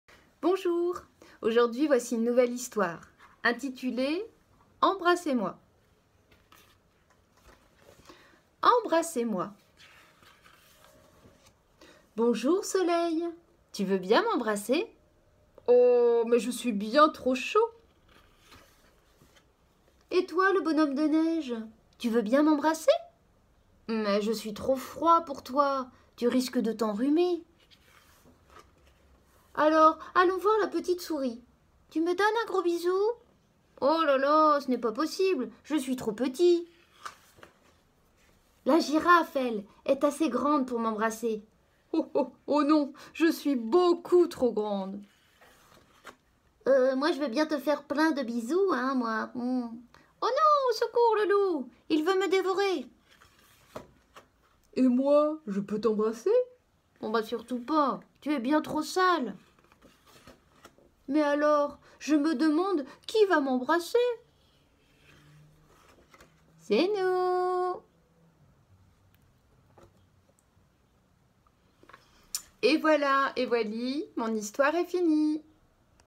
[Lecture] Embrassez-moi
Aujourd'hui, Ingrid, Éducatrice de jeune enfant chargée de l’éveil culturel au sein des crèches de la ville de Tours, vous propose la lecture d'un album intitulé "Embrassez-moi".